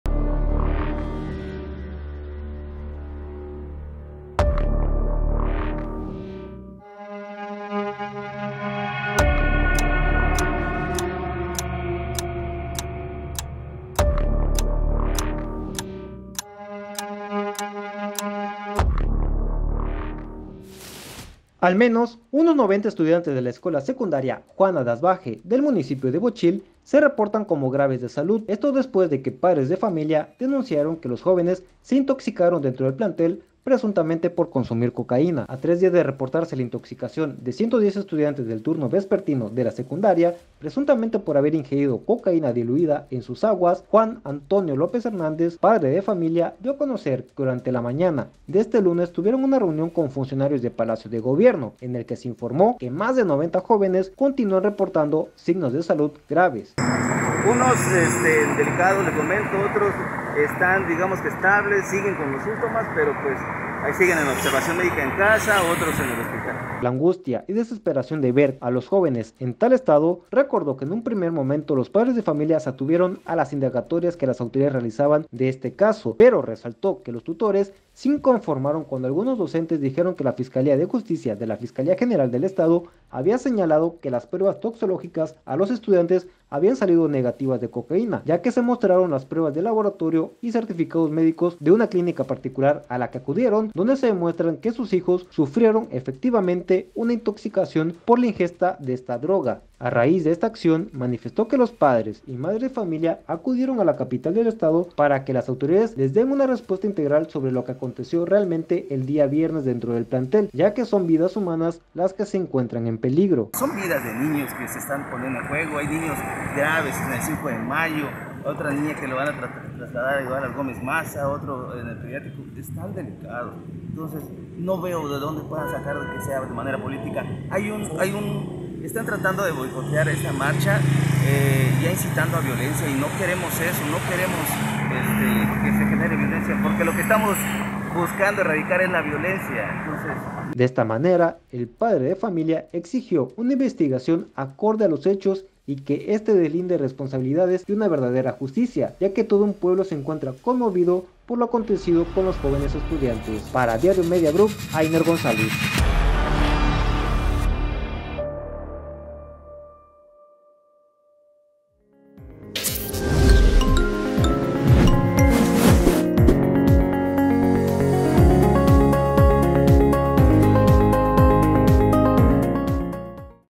90 estudiantes de Bochil graves por intoxicación con cocaína
👩🏻👦🏻🏫CÁPSULA | El día viernes 7 de octubre, padres de familia denunciaron que al menos 110 estudiantes de esta secundaria fueron hospitalizados por una intoxicación masiva con cocaína que habría sido diluida en sus bebidas.

Con información de Ainer González / Diario de Chiapas